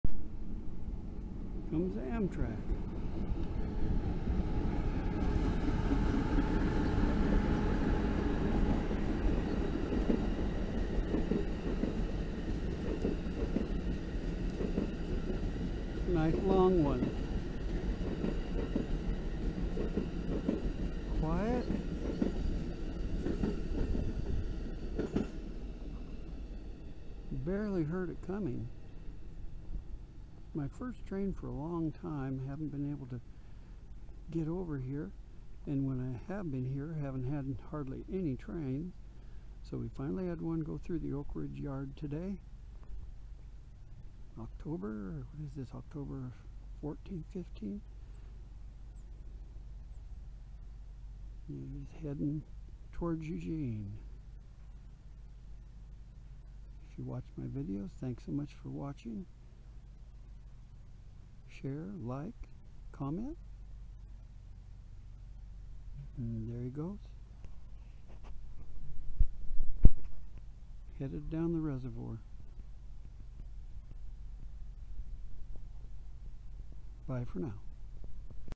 Gliding Through
Amtrak coming down the hill heading north to Eugene.